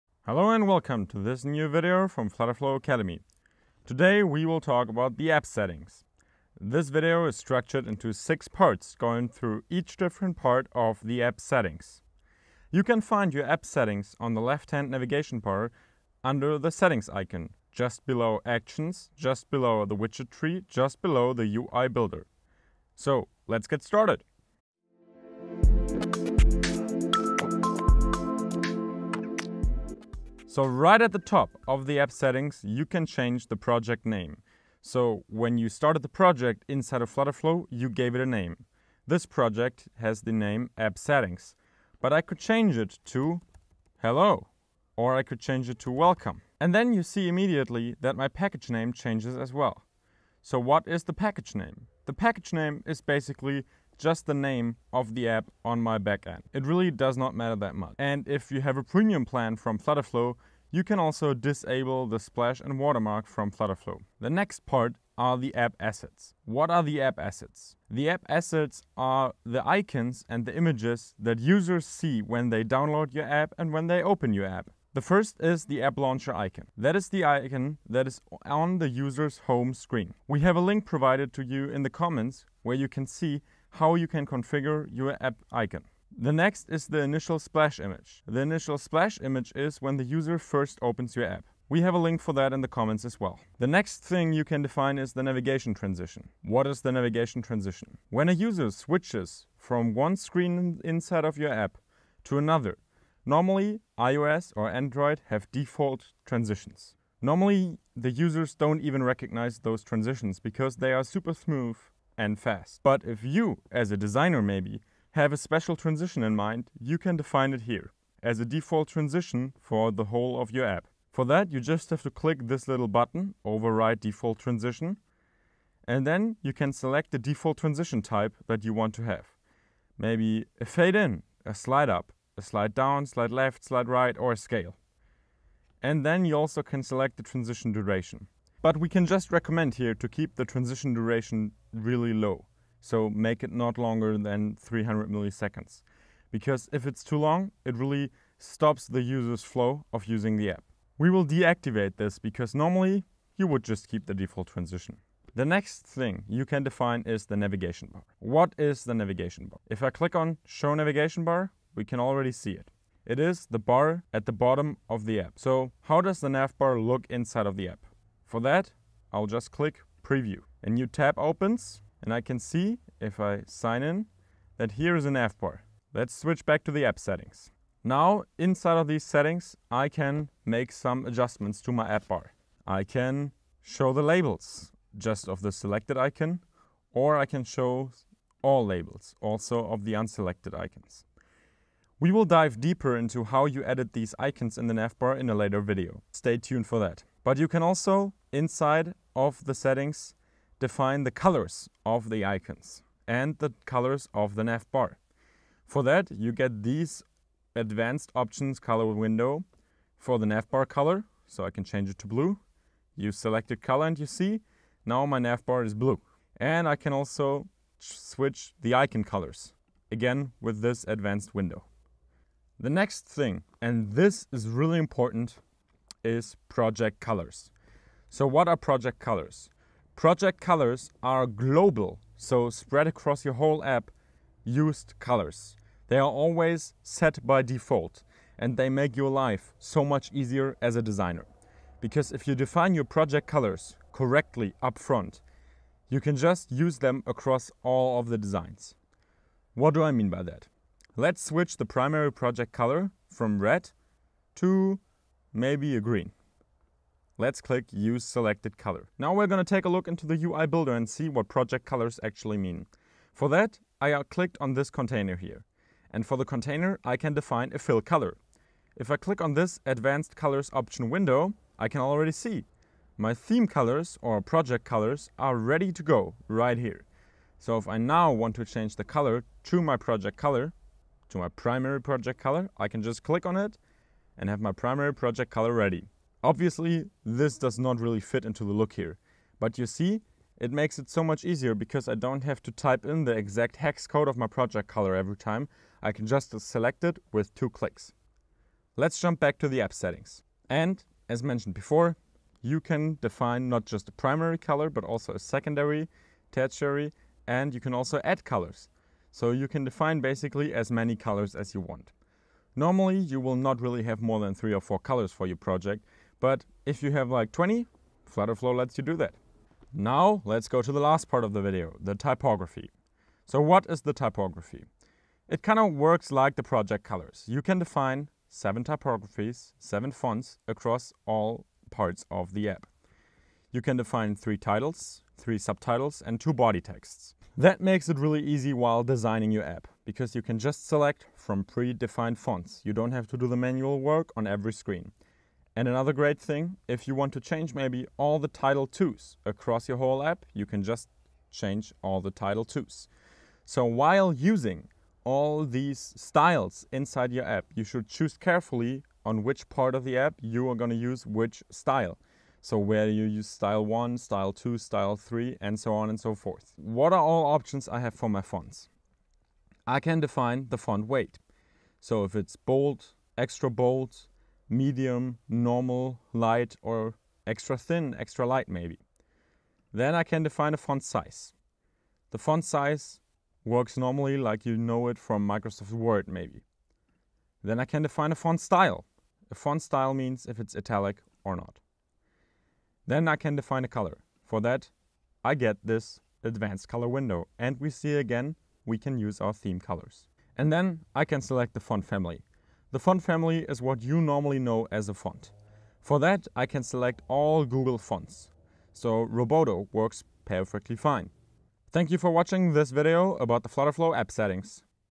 AppSettings | Intro to FlutterFlow | FlutterFlow Academy
This Video is about the App Settings Tab inside of FlutterFlow! It is a Video from our intro to FlutterFlow Series!

Parts:
0:00 Intro
0:31 Naming
1:04 App Assets
1:33 Navigation Transition
2:35 Nav Bar
3:43 Project Colors
5:20 Typography